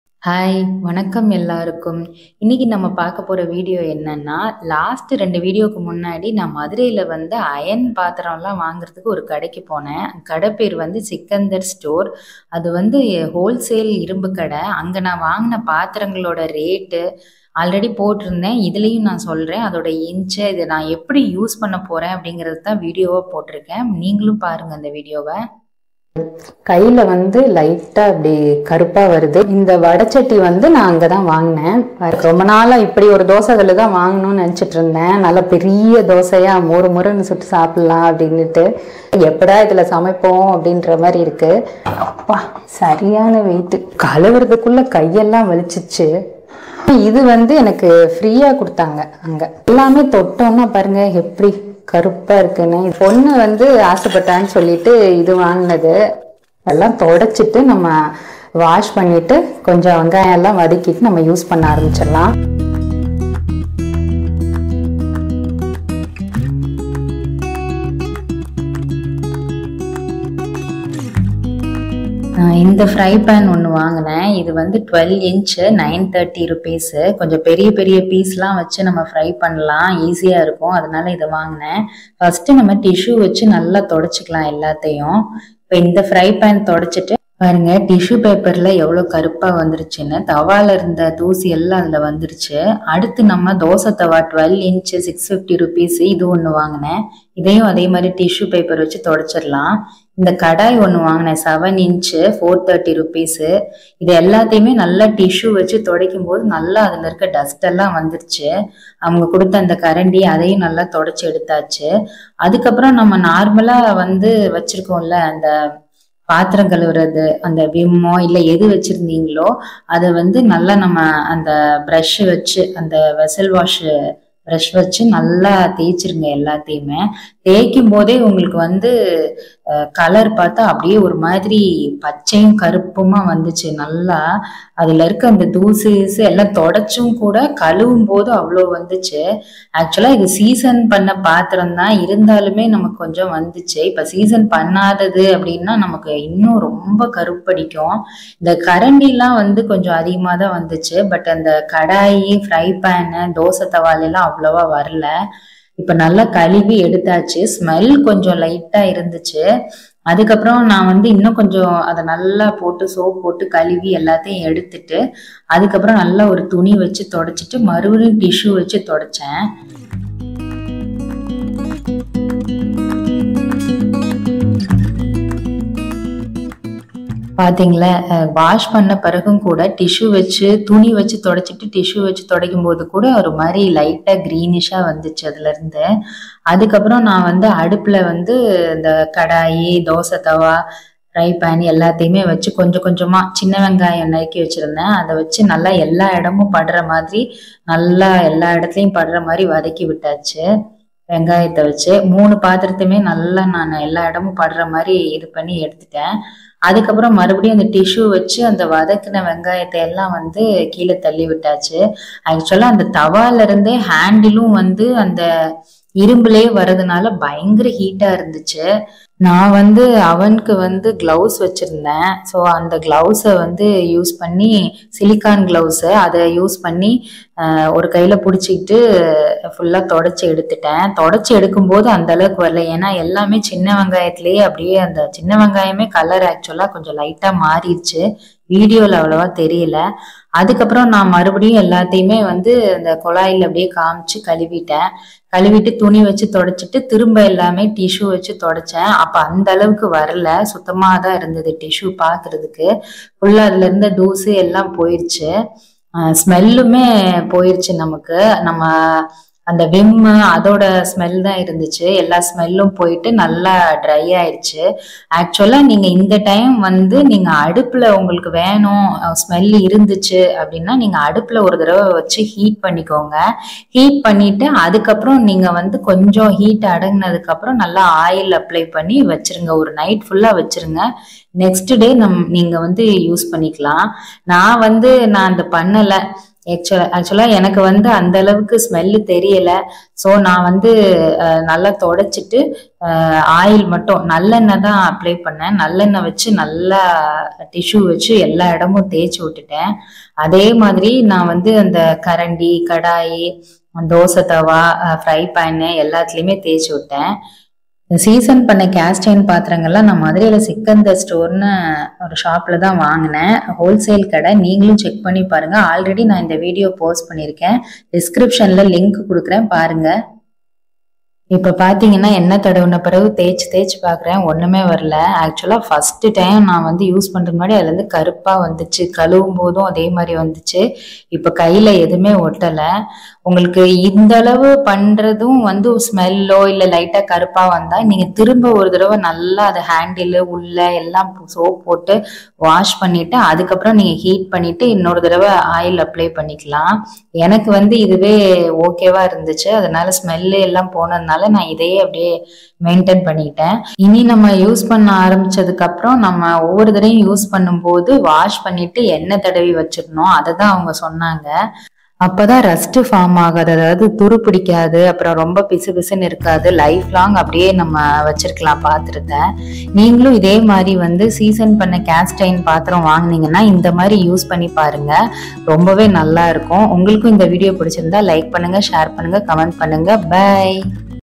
புது இரும்பு பாத்திரங்களை பழக்குவது எப்படி|how to use new season cast iron for the first time
Sikkandar stores Madurai
online order for whatsapp 👇